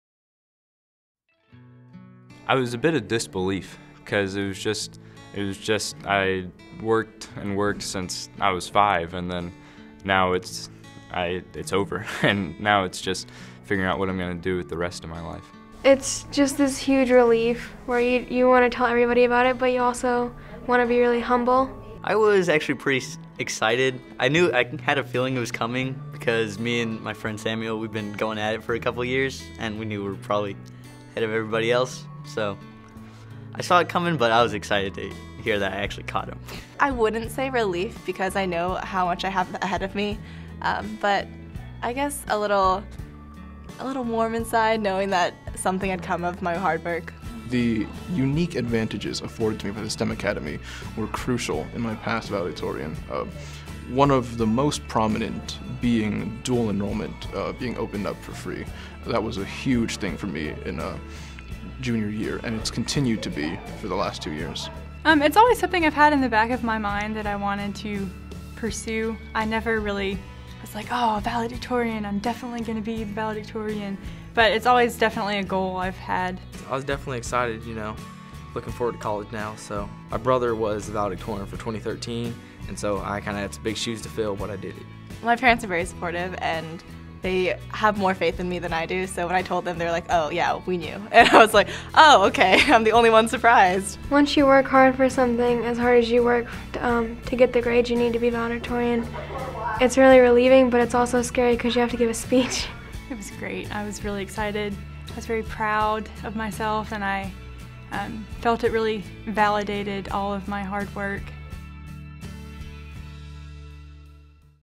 2015 Valedictorians
The Class of 2015 Valedictorians and Salutatorians were recognized at the week’s board meeting. We sat down with the Valedictorians to find out what it was like when they found out that their hard work had paid off. You may remember David Zhang from Kenwood from our spotlight last year

Unfortunately, representatives from Northeast High School were unable to attend.